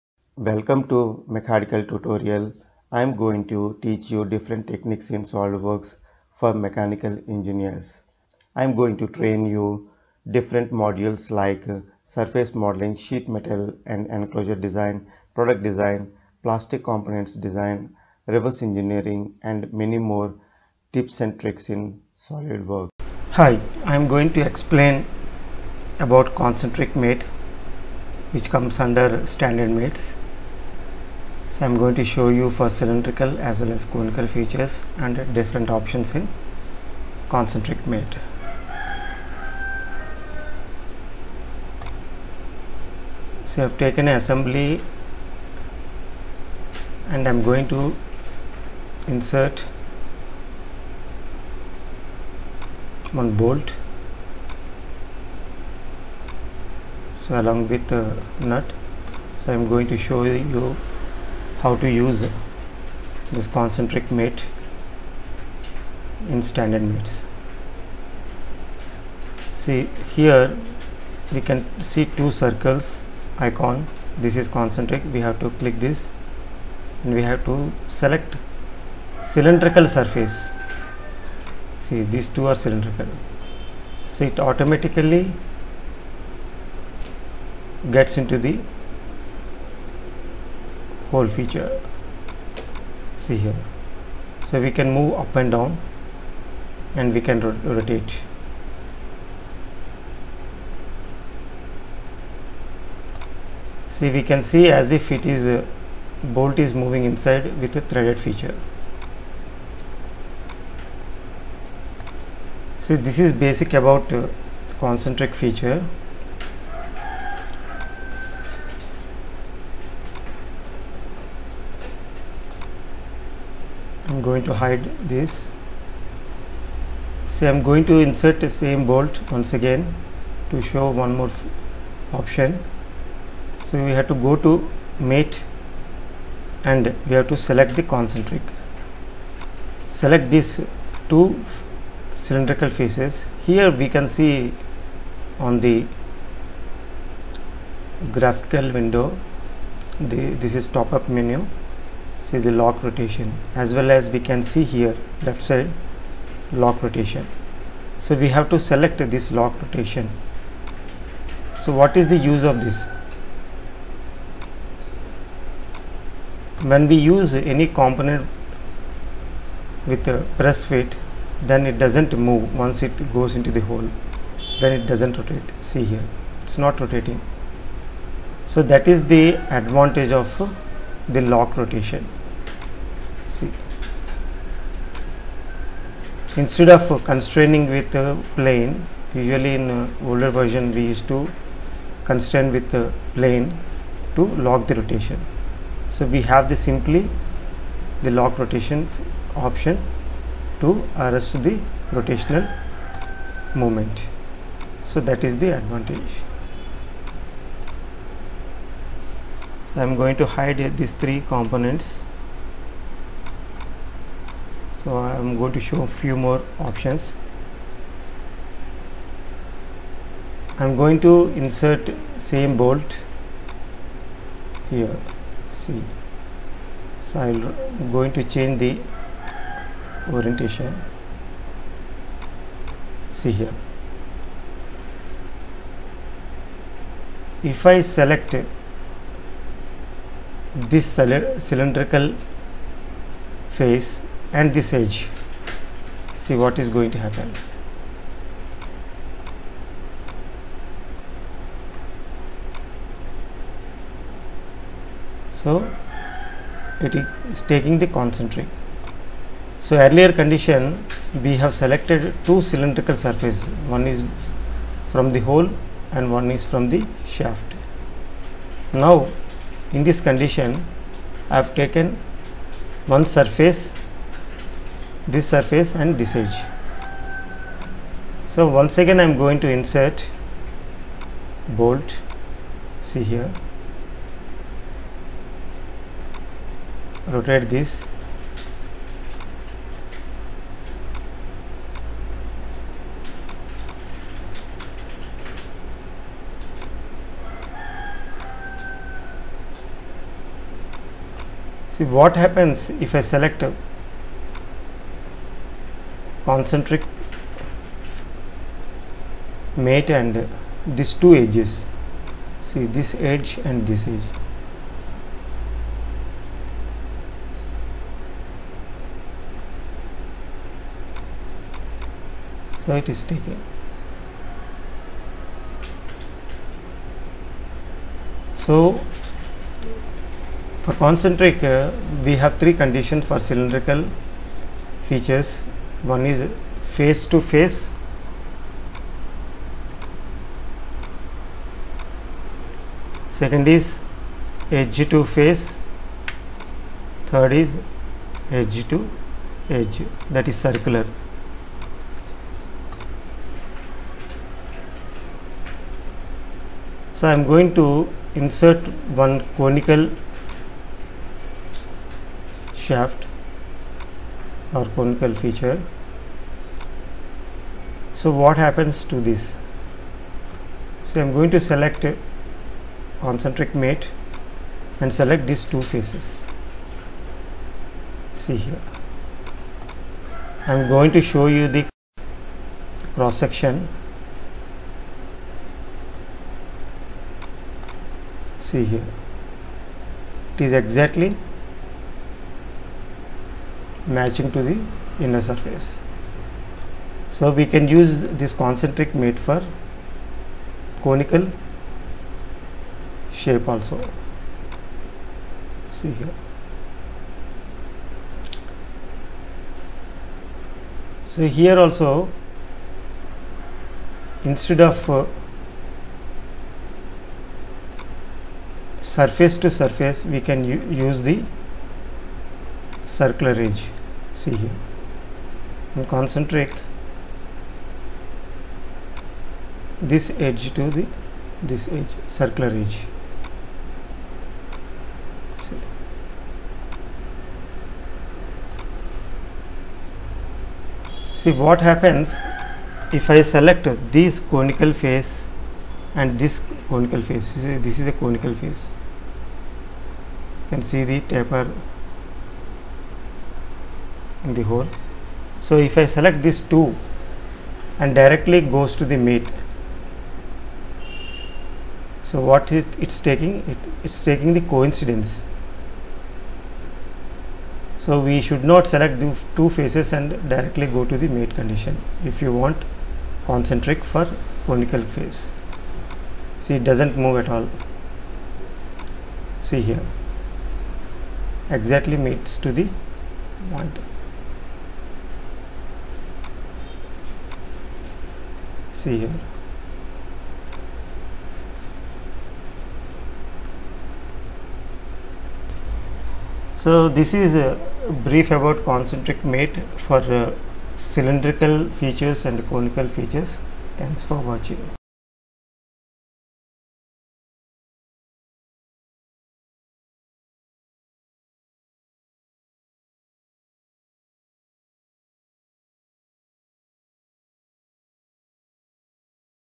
Concentric Mate In SolidWorks
In this video, I am going to explain about "Concentric Mate" in Solidworks.

Concentric Mate is used when, Concentric centers cylindrical features together: pins to holes, holes to holes, tapered shafts to tapered holes, etc. The Lock Rotation option is for when you are lining up press fit cylindrical features, fasteners, hubs fixed with a set screw, or other features you don’t intend to be free to rotate.